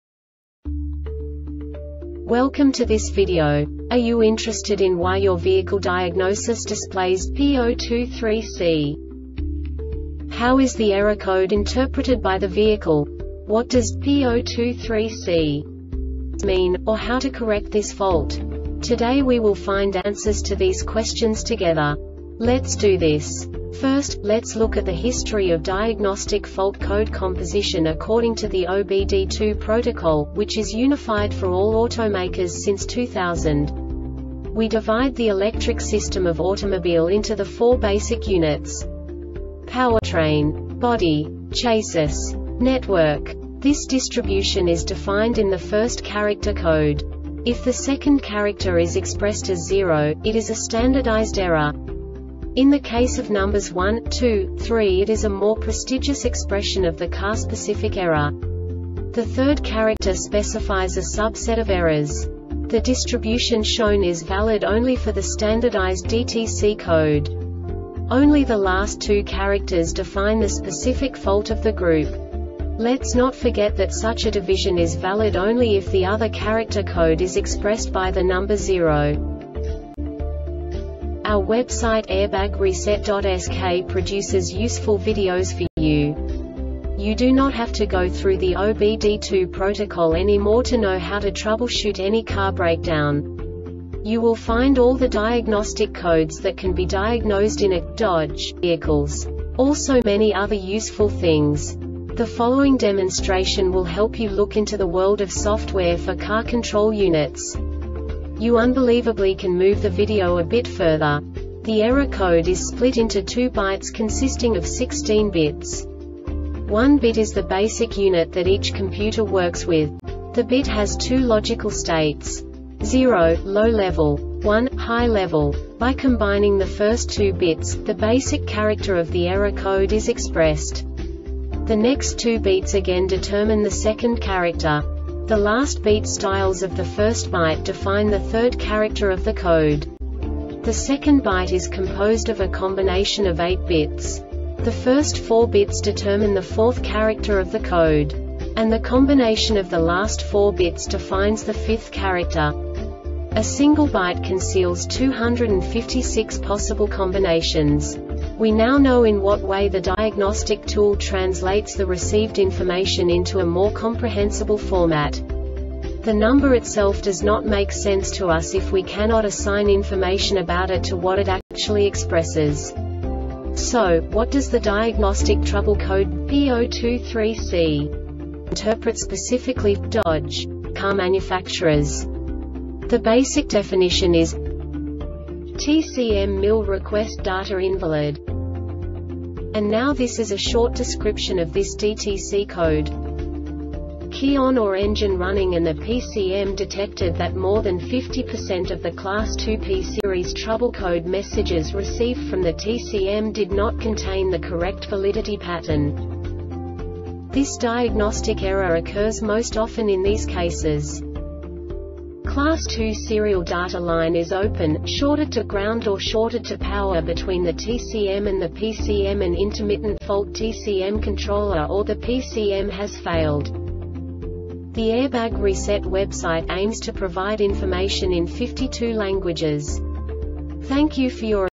DTC Dodge P023C Short Explanation
Contents:
0:21 Basic DTC analysis according to OBD2 protocol standard.
1:48 Insight into programming
2:58 A diagnostically understandable description of the Diagnostic Trouble Code
3:05 Basic error definition

Make:
Dodge
Code:
P023C
Definition:
TCM MIL Request Data Invalid
Description:
Key on or engine running;and the PCM detected that more than 50% of the Class 2 P-Series trouble code messages received from the TCM did not contain the correct validity pattern.
Cause:
Class 2 serial data line is open,shorted to ground or shorted to power between the TCM and the PCM (an intermittent fault)TCM (controller) or the PCM has failed